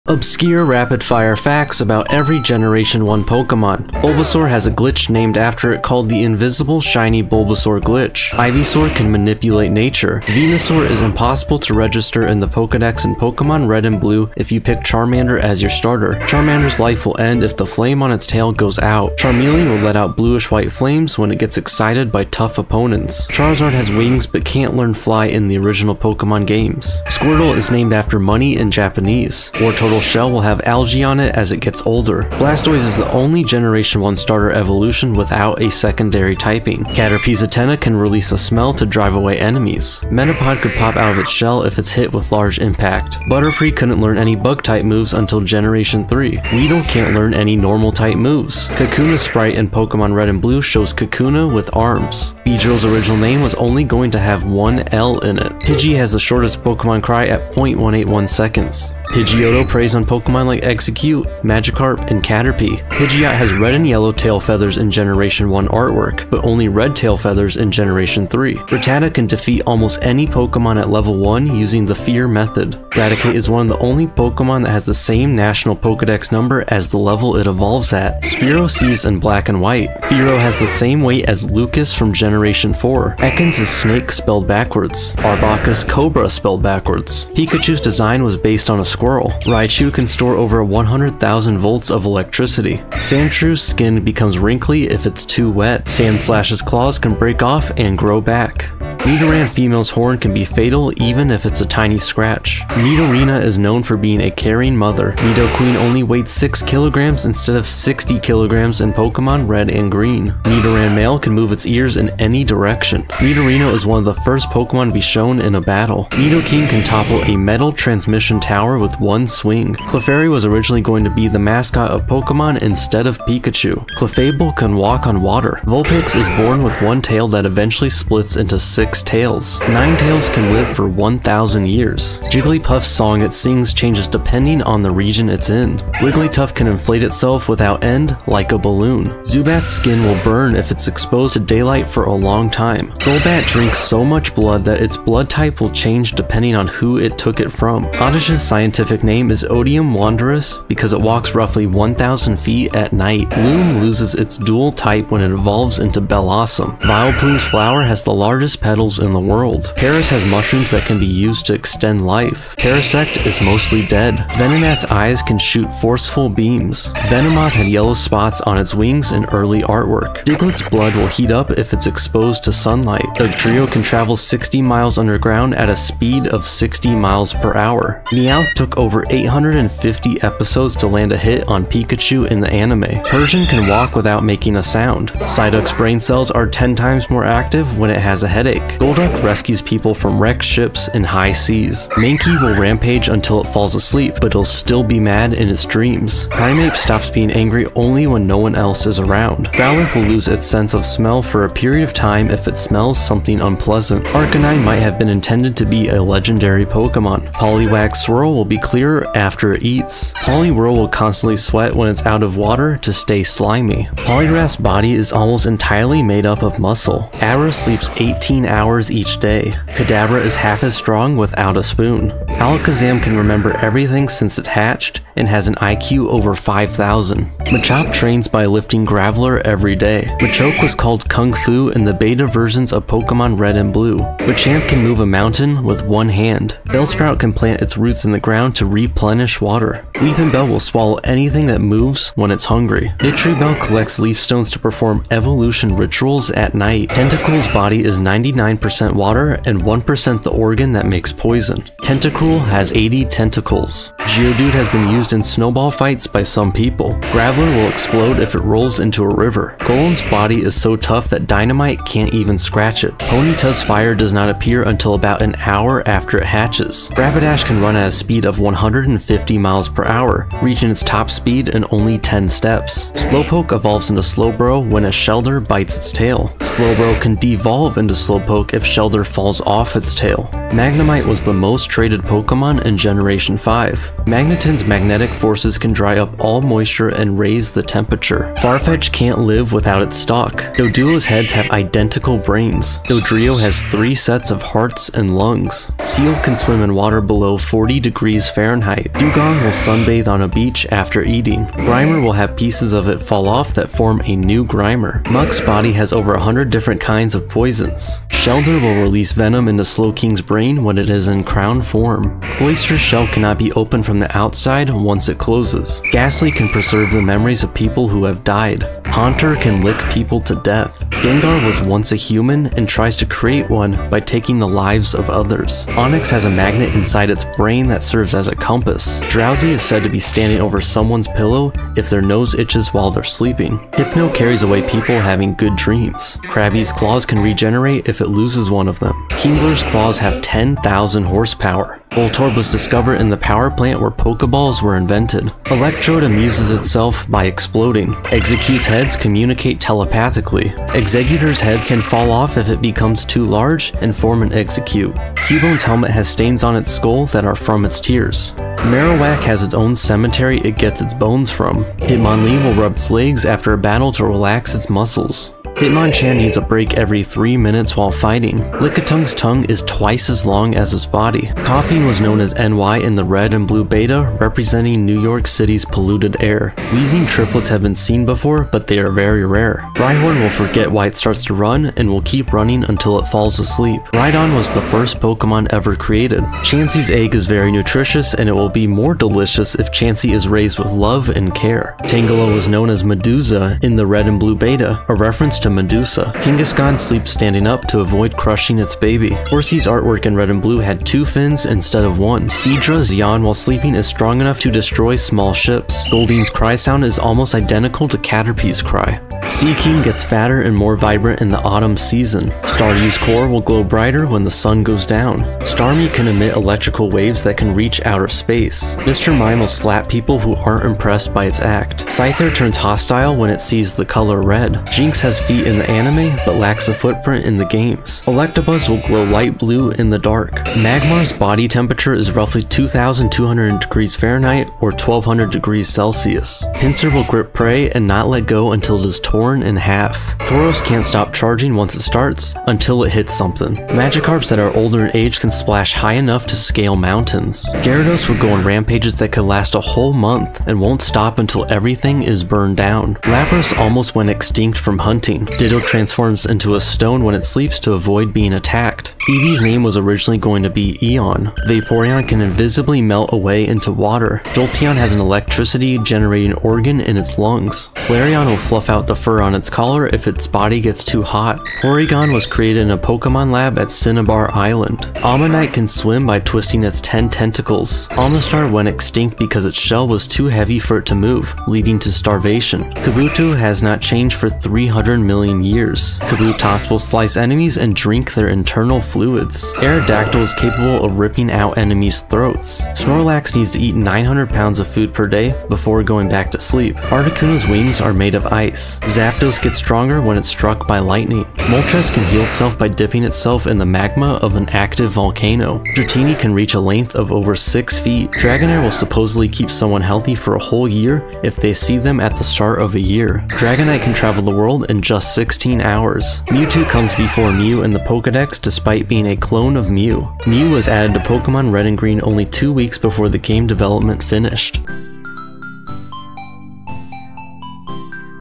Obscure Facts About EVERY Generation 1 Pokemon
Did you know these obscure random facts about every Pokemon from the original generation 1 about all 151 Pokemon? In this video, I provide rapid fire facts about each Pokemon in the first generation! Some of these facts are from Pokédex entries, the Pokemon anime, and the video game series!

Credits for Animated Pokemon Sprites:
Furret Turret @TilableToast's Twitter

Many of these facts were discovered from Bulbapedia!